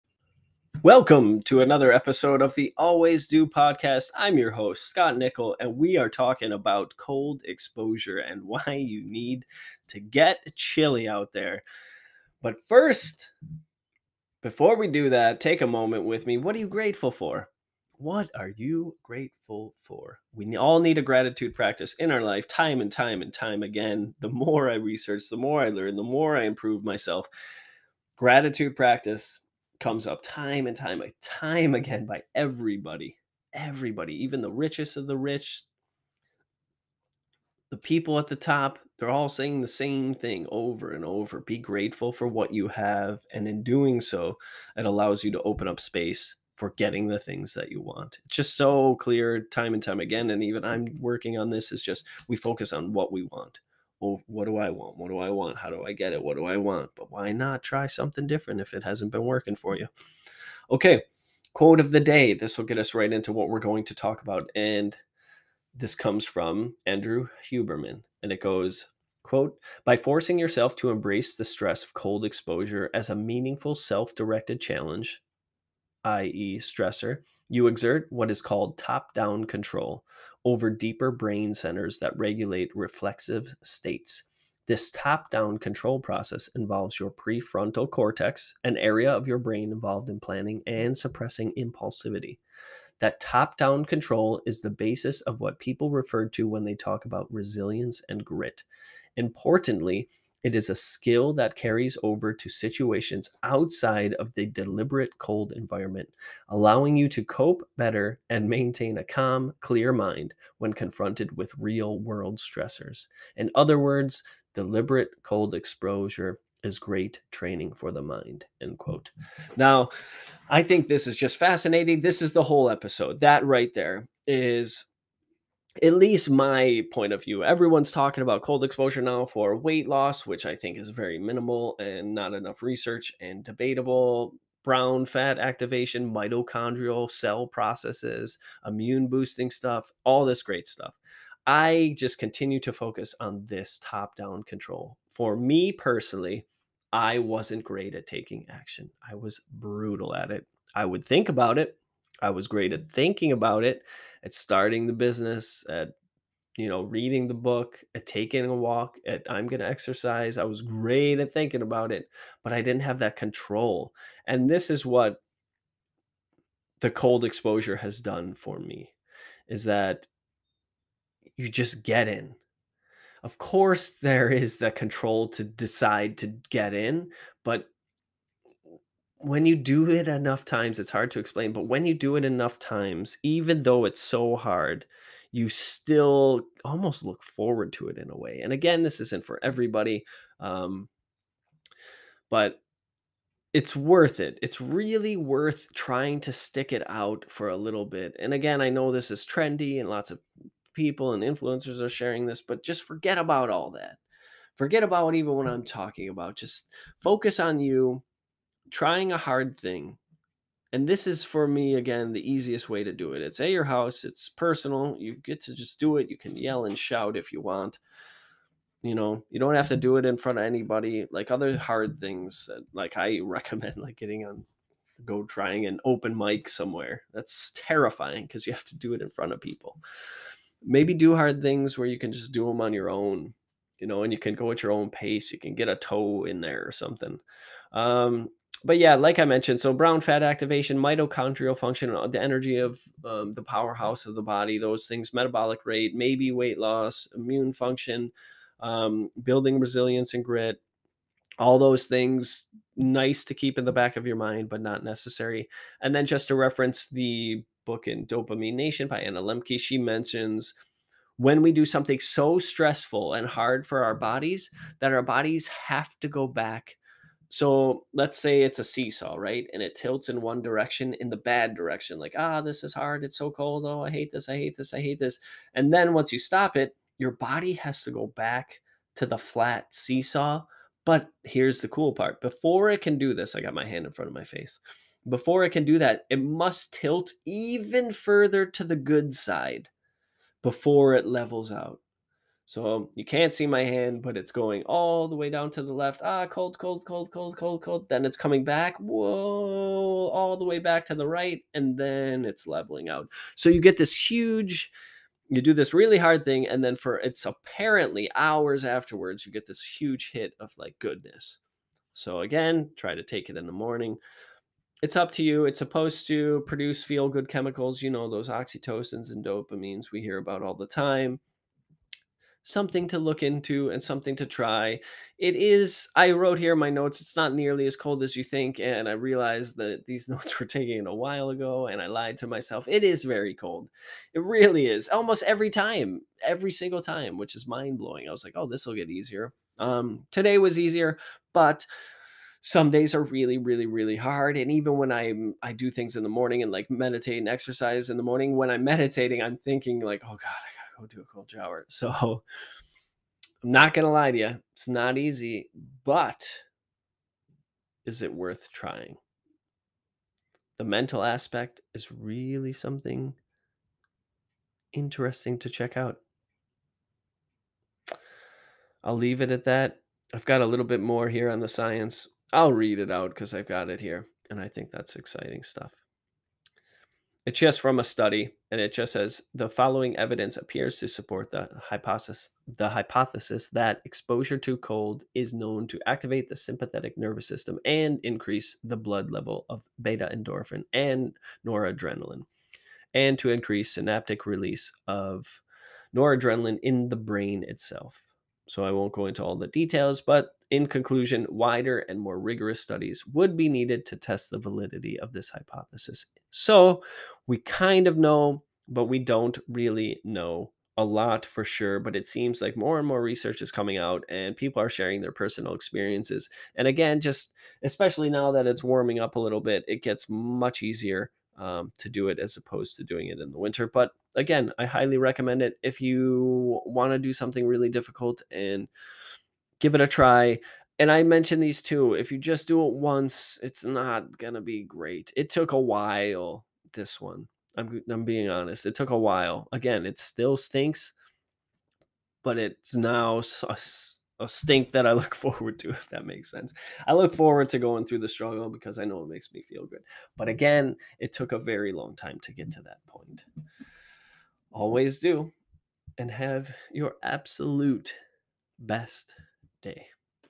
Ep. 37: Cold Exposure and Why You Need To Do It
By forcing yourself to embrace the stress of cold exposure as a meaningful self-directed challenge (i.e., stressor), you exert what is called ‘top-down control’ over deeper brain centers that regulate reflexive states. This top-down control process involves your prefrontal cortex – an area of your brain involved in planning and suppressing impulsivity. That ‘top-down’ control is the basis of what people refer to when they talk about “resilience and grit.” Importantly, it is a skill that carries over to situations outside of the deliberate cold environment, allowing you to cope better and maintain a calm, clear mind when confronted with real-world stressors. In other words, deliberate cold exposure is great training for the mind.